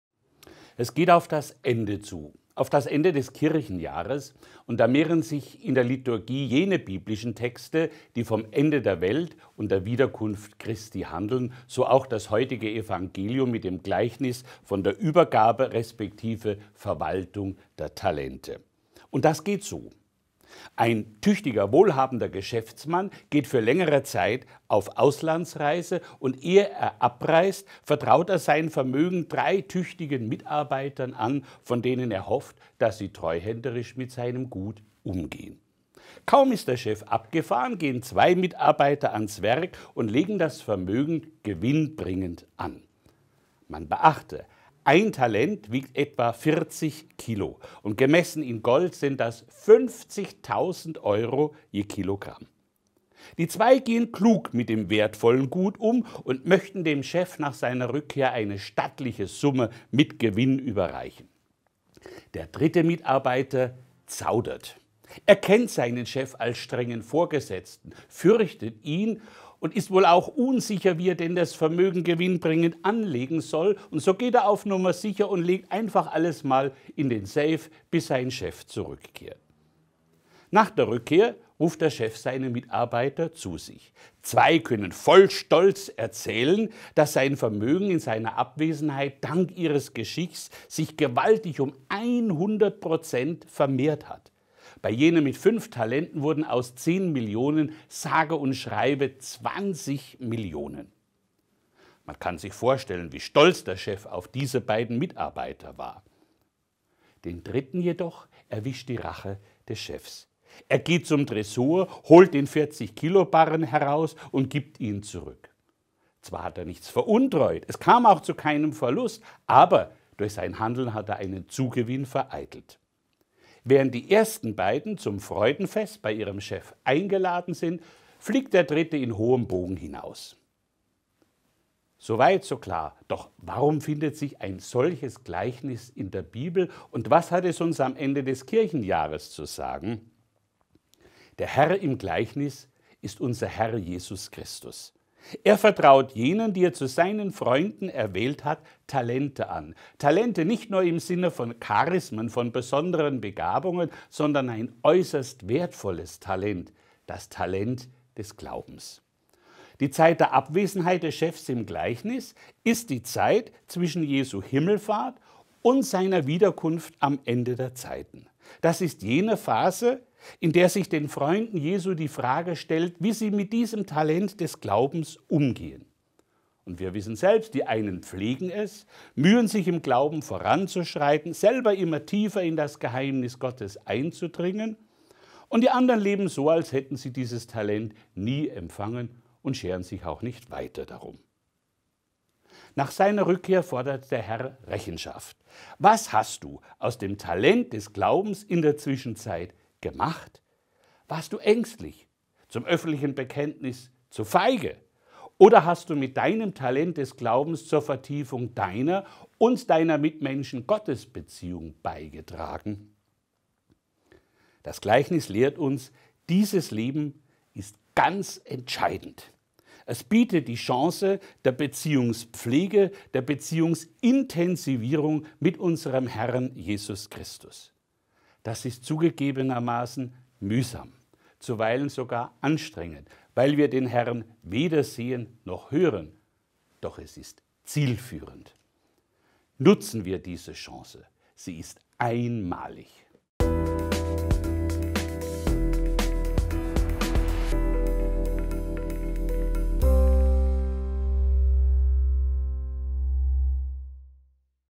Chance deines Lebens! - Predigt zum 15. November
Beziehung leben, pflegen und intensivieren sollten wir nicht nur mit Mitmenschen, sondern als Christen vor allem auch mit Gott. Eine einmalige Chance dazu bietet unser Leben, im Hier und Jetzt, jeden Tag. Das Leben als Chance ist das Thema von Dr. Bernhard Kirchgessner, Leiter des Exerzitien- und Bildungshauses Spectrum Kirche Passau, in seiner Predigt zum 33. Sonntag im kirchlichen Jahreskreis am 15. November 2020.